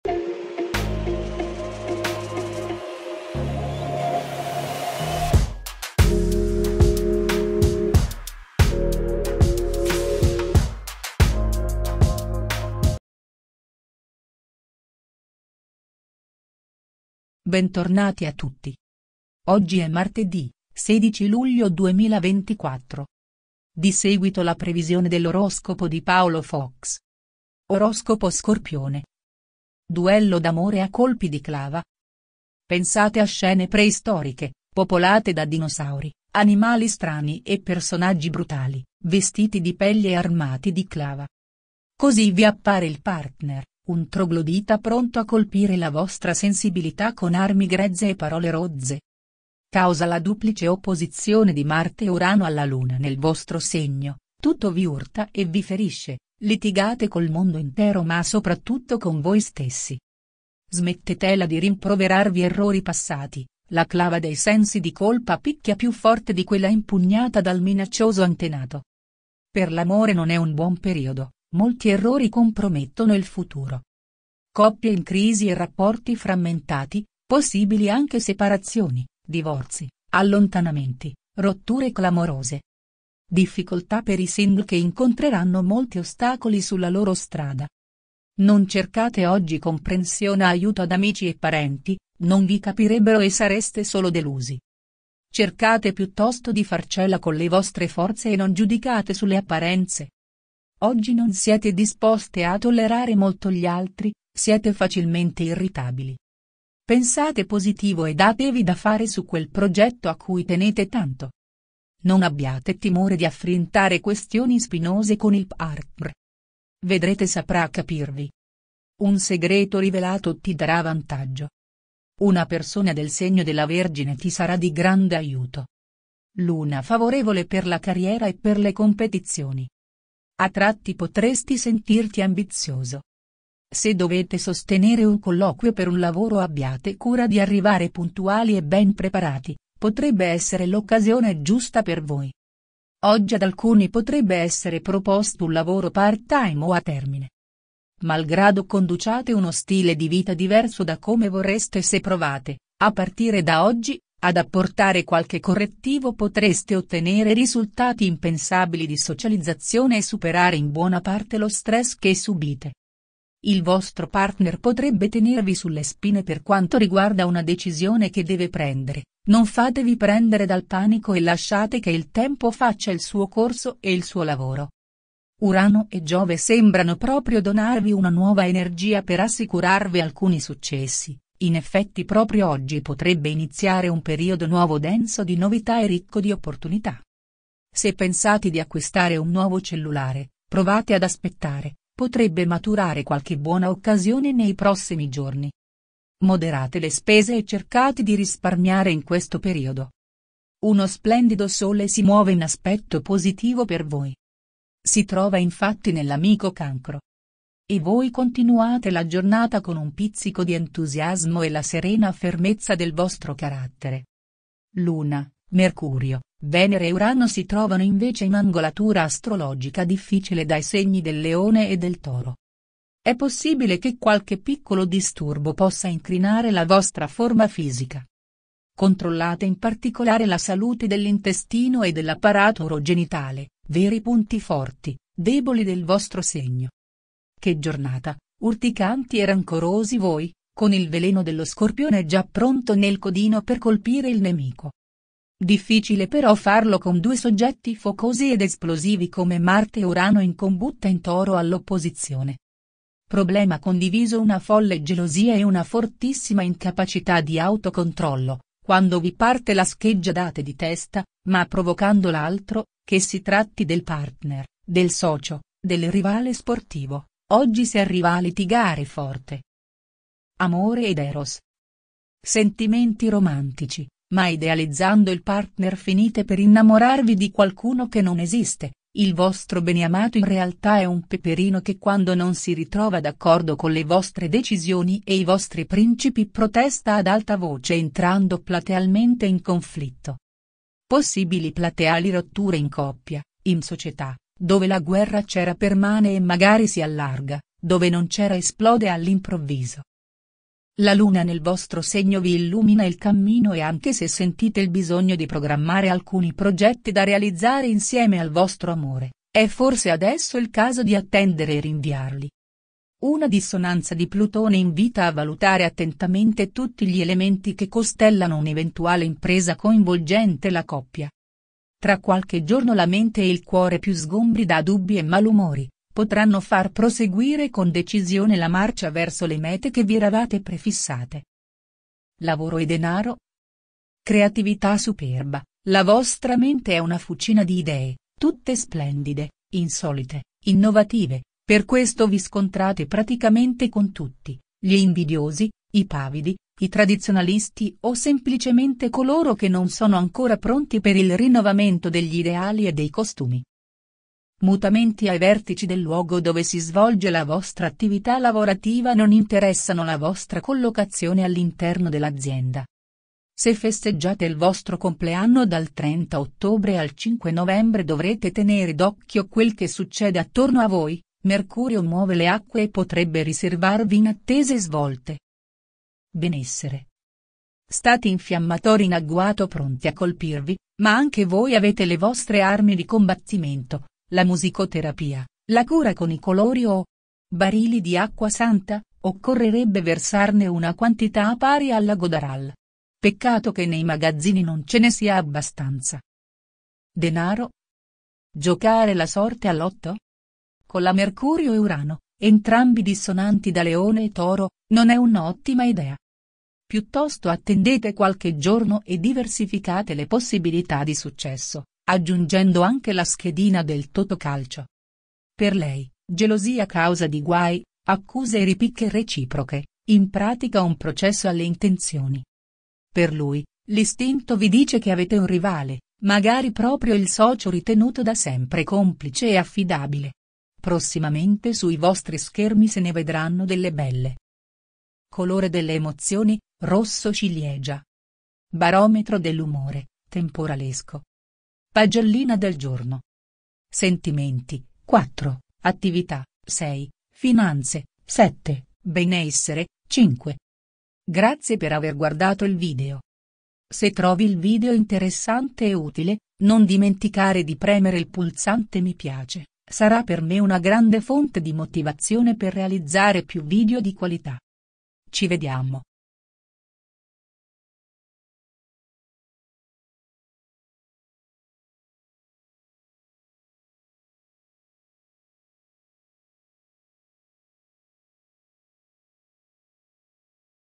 Scorpione ♏️ L'oroscopo di Paolo Fox - 16 Luglio 2024 - Duello d’amore a colpi di clava
The astrologer Paolo Fox presents the 2024 horoscope with the favors of the stars for the twelve signs of the zodiac.